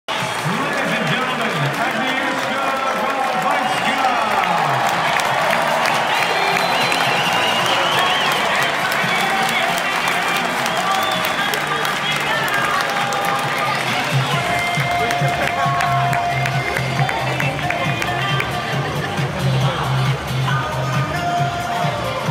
Miami Open - March 24, 2018
Quick videos of the main Stadium for Miami Open. This is Crandon park at Key Biscayne, last year before moving to the Hard Rock Stadium